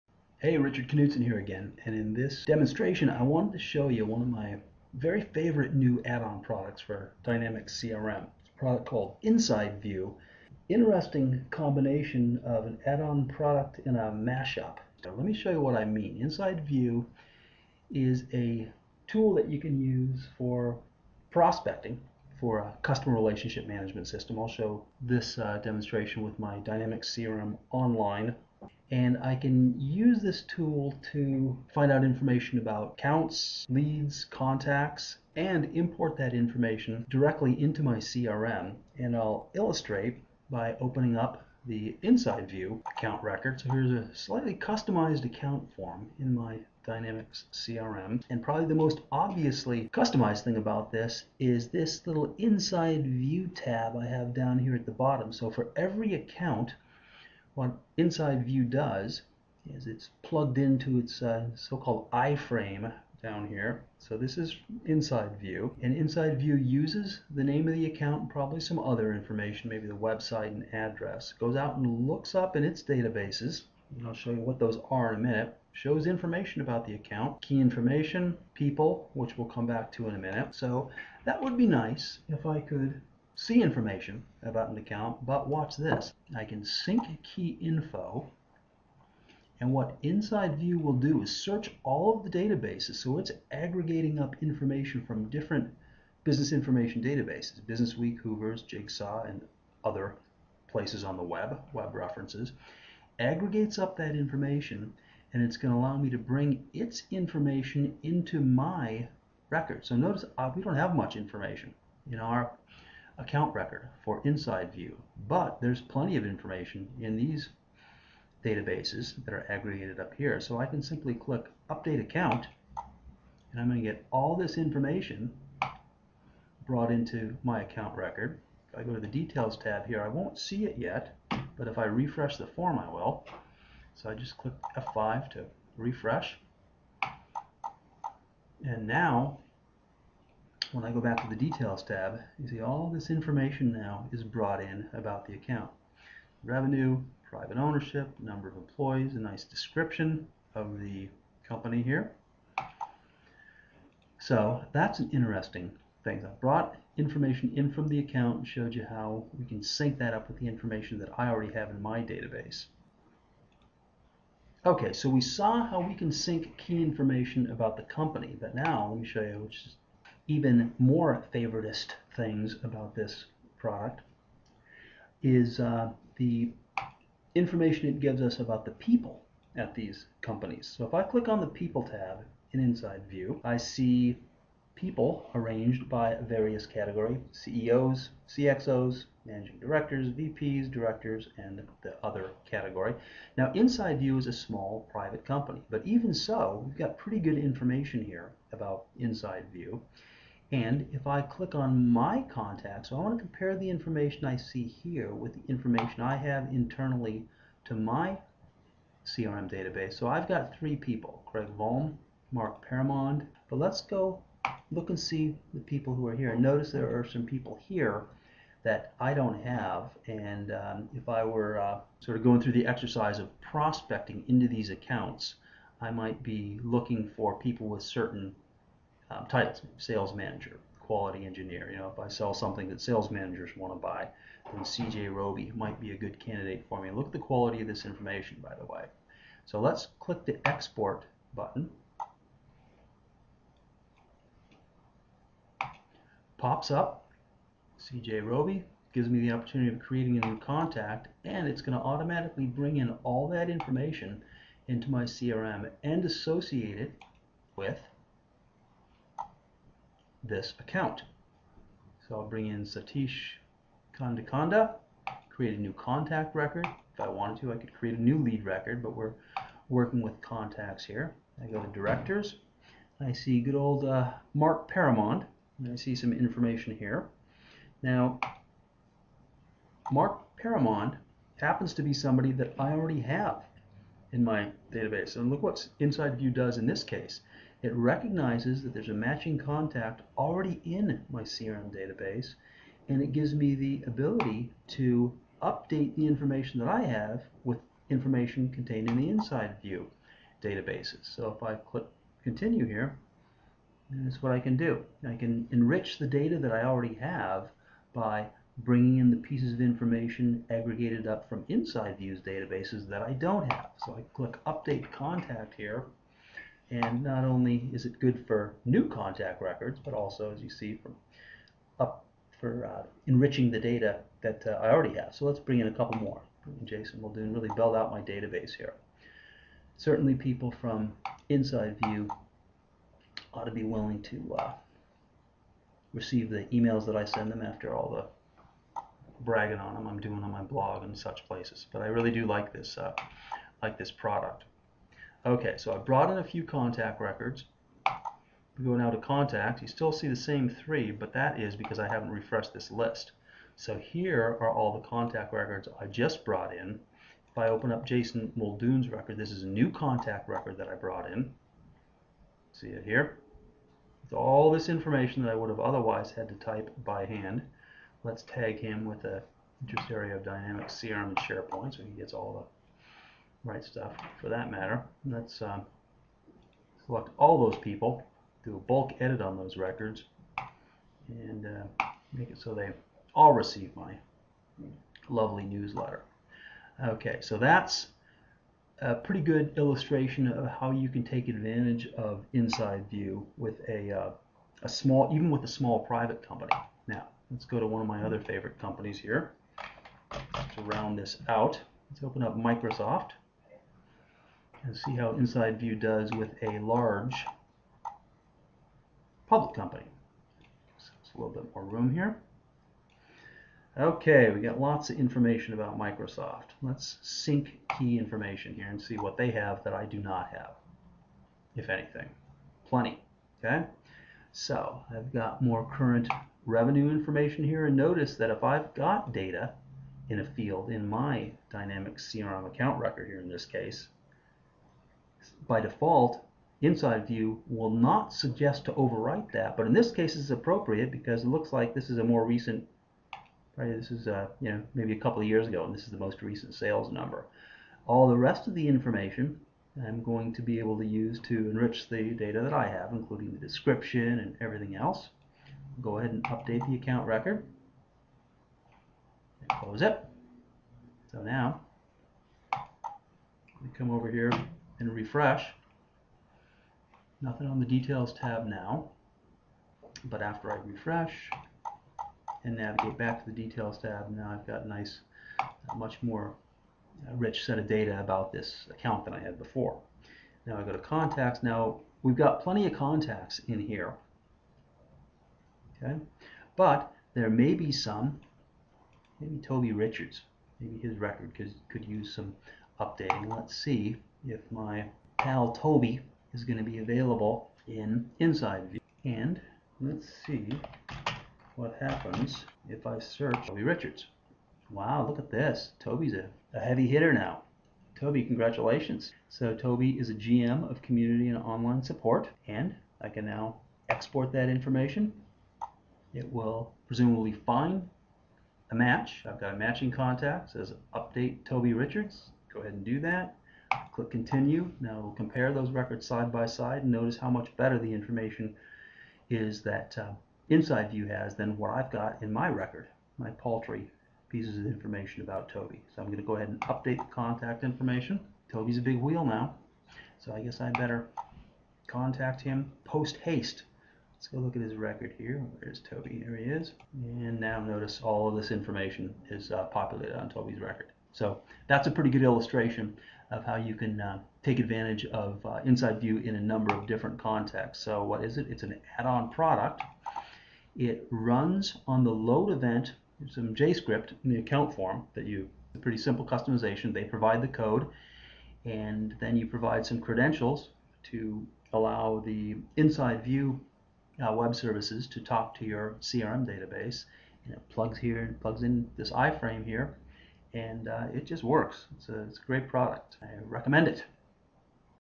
Inside View -- the Ultimate CRM Mashup
If you ever need to prospect accounts and leads, or if you ever need to improve the quality of data in your CRM, InsideView can be an indispensable tool. They've recently added an integration with Dynamics CRM that has to be seen to be believed. This demo shows how you can automatically import contacts from their aggregated database, and how you can use their data to improve the quality of yours.